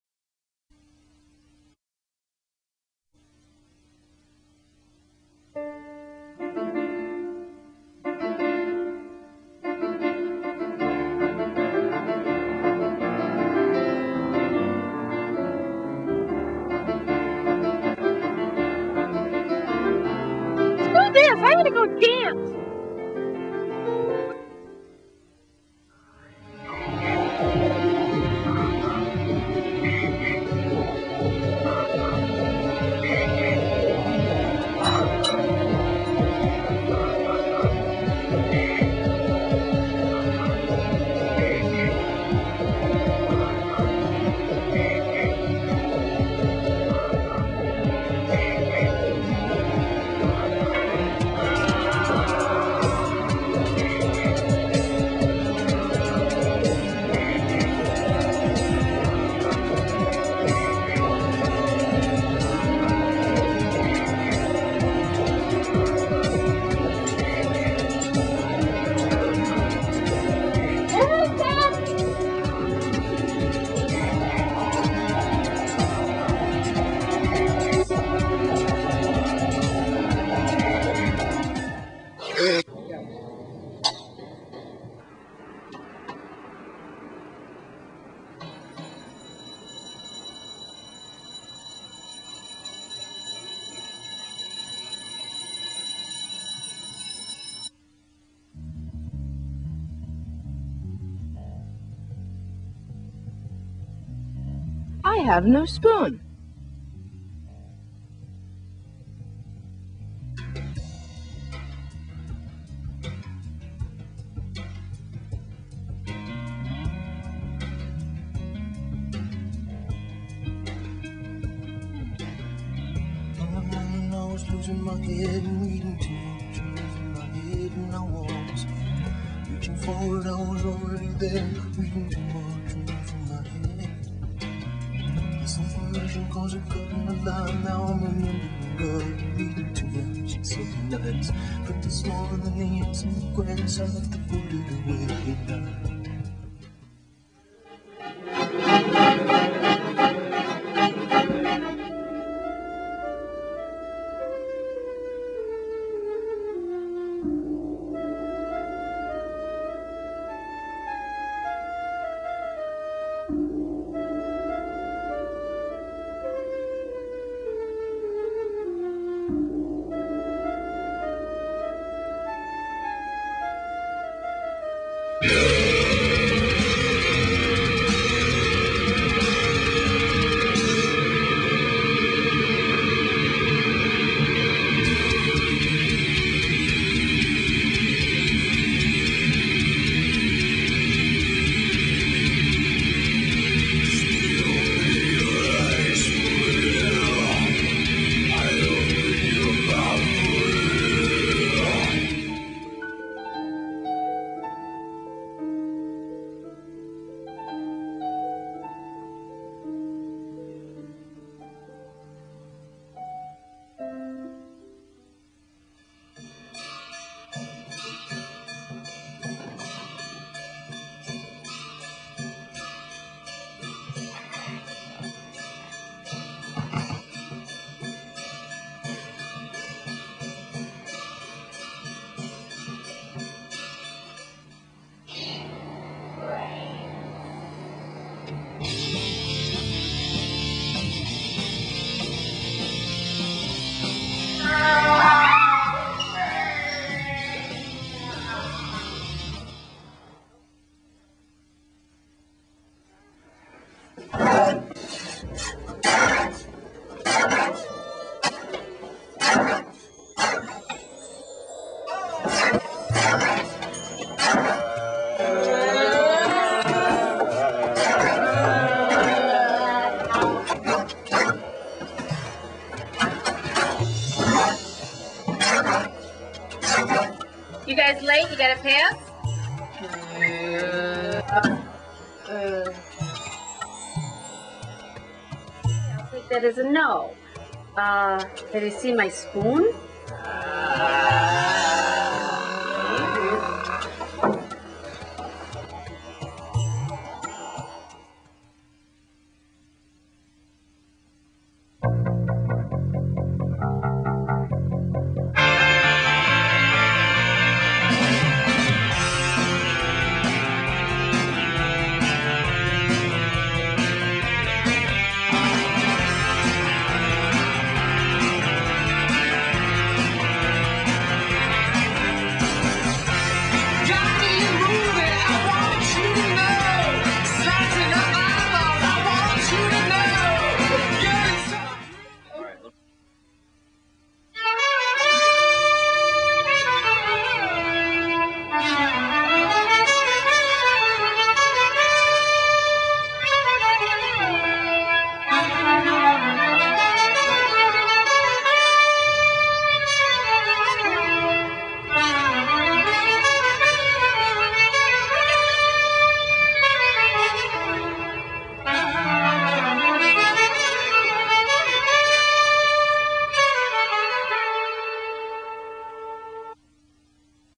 Attack of the Killer Spoon
A really super awesome student film I did in my freshman year of high school.